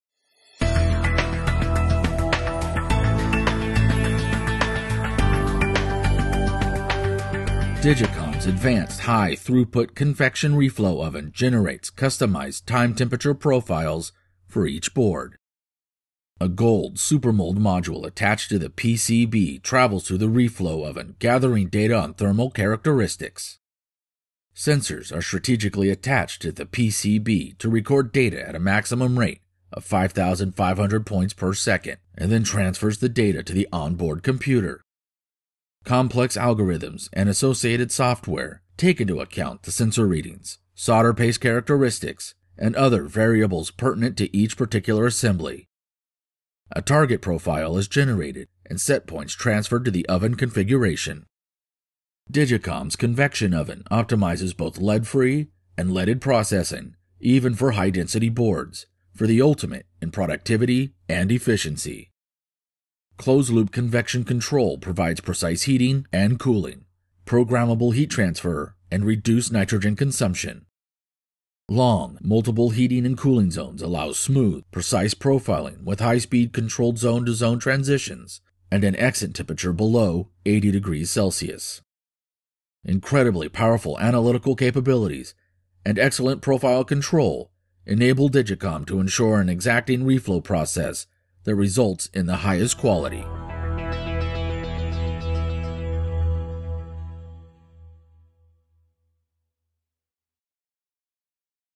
Solder Reflow Services from Digicom
Digicom's use of precision profiling results in the highest quality during the soldering process, since the temperature is controlled precisely at each zone.